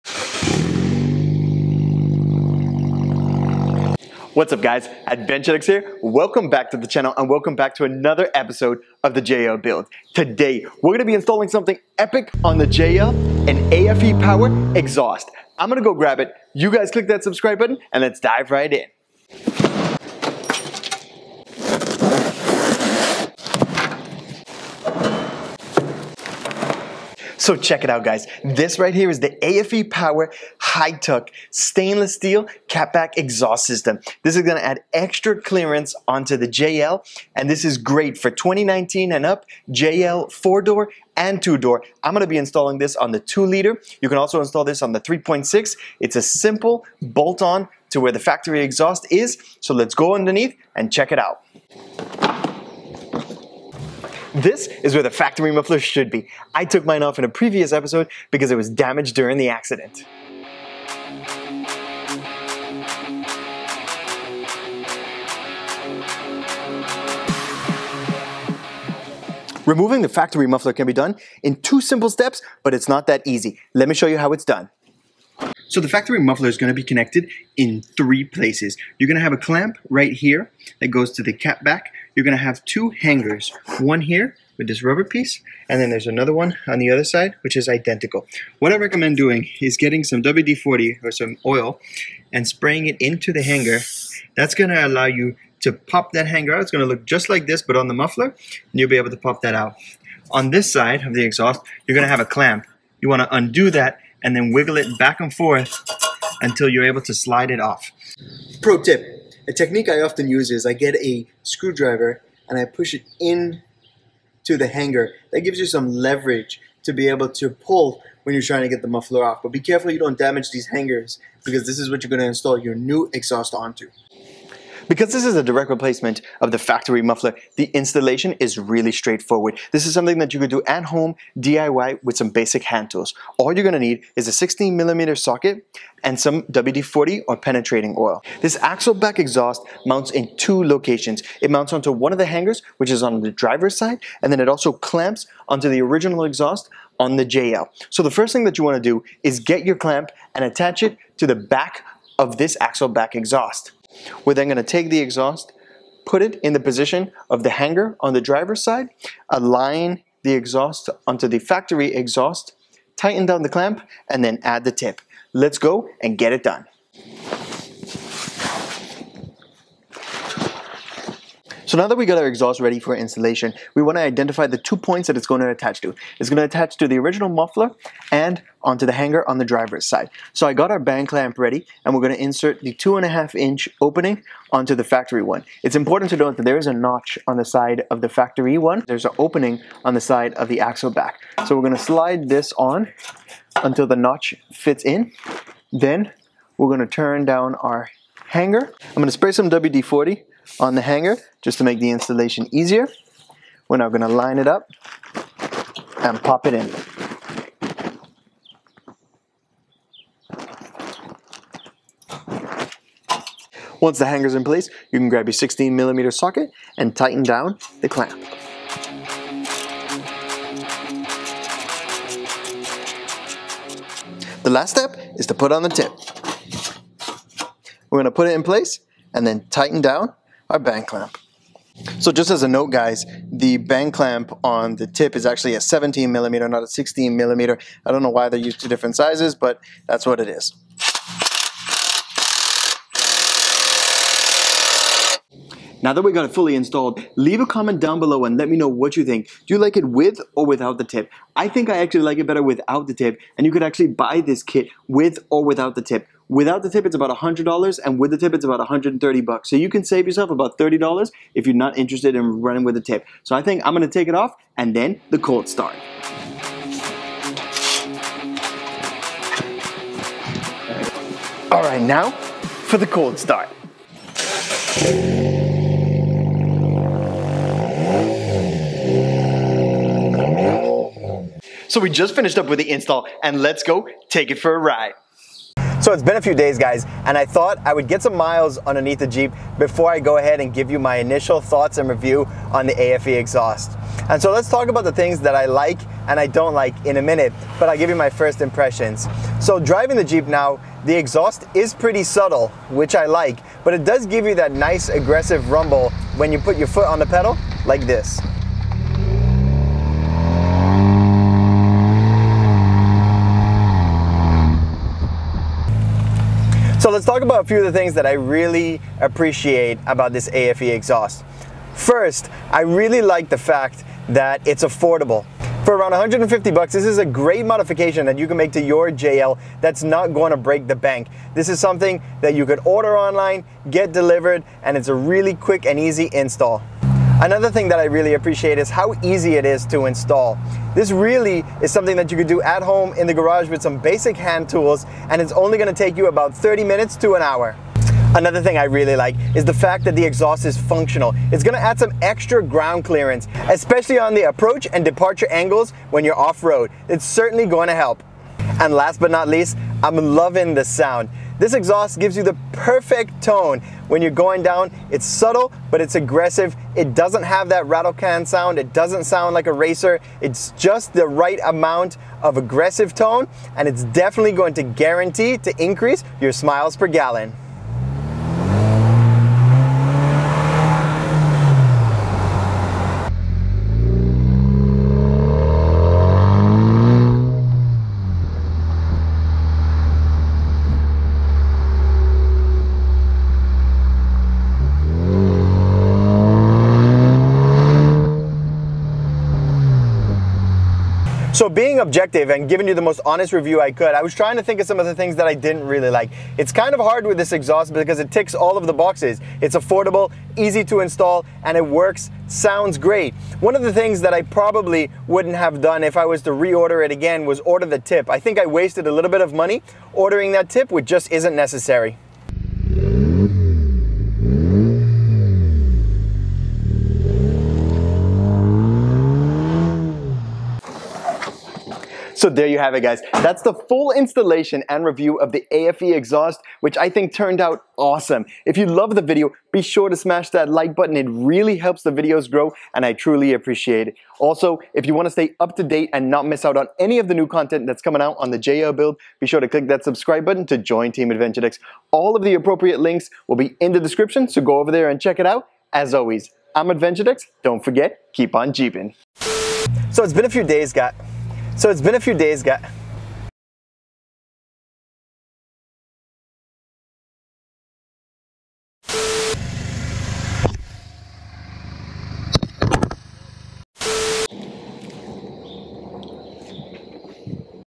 How to install a Jeep Exhaust! - 3 Easy Steps!
In this video, I will be installing a aFe Power Exhaust on my 2019 Jeep Wrangler JL. This installation tutorial show you how yo install a Jeep Exhaust in a few easy steps!

aFe Power Jeep Exhaust

My Jeep Build List

Camera Used